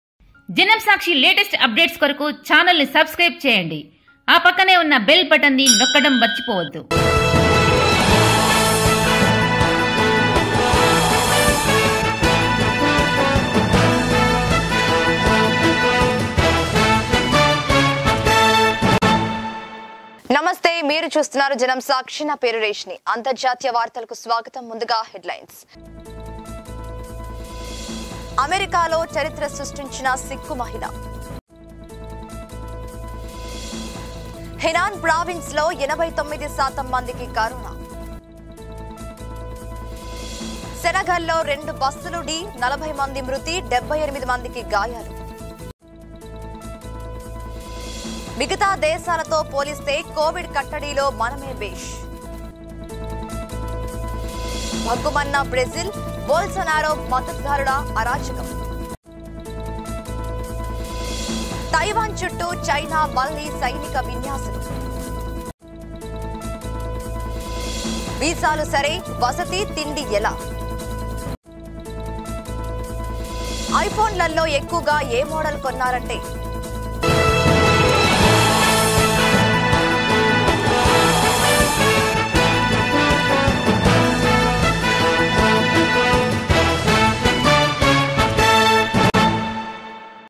అంతర్జాతీయ వార్తల ముఖ్యాంశాలు || international news headlines
1. అమెరికాలో చరిత్ర సృష్టించిన సిక్కు మహిళ
2.  హెనాన్‌ ప్రావిన్స్‌లో 89 శాతం మందికి కరోనా
3. సెనగల్‌లో రెండు బస్సులు ఢీ.. 40 మంది మృతి, 78 మందికి గాయాలు
4. మిగతా దేశాలతో పోలిస్తే.. కొవిడ్ కట్టడిలో మనమే భేష్
5. భగ్గుమన్న బ్రెజిల్‌.. బోల్సొనారో మద్దతుదారుల అరాచకం
6. తైవాన్ చుట్టూ చైనా మళ్లీ సైనిక విన్యాసాలు
7. వీసాలు సరే.. వసతి, తిండి ఎలా..?
8. ఐఫోన్లలో ఎక్కువగా ఏ మోడల్ కొన్నారంటే..
9. విదేశాల్లో ఉంటున్న మనోళ్ల గురించి కొన్ని ఇంట్రెస్టింగ్ ఫ్యాక్ట్స్..!
10.  అరుదైన రాబందు ప్రత్యక్షం.. కాన్పూర్‌ అటవీ అధికారులకు అప్పగింత

1. A Sikh woman who made history in America
2. 89 percent of people in Henan province have corona
3. Two buses collided in Senegal.. 40 people died and 78 people were injured
4. Compared to other countries.. we are the worst in the fight against Covid
5. Bhaggumanna Brazil.. Anarchy of Bolsonaro supporters
6. China again military maneuvers around Taiwan
7. Visas ok.. how about accommodation, food..?
8. Which model of iPhones are bought the most?
9. Some interesting facts about Manola living abroad..!
10. Rare vulture sighting.. Handover to Kanpur forest officials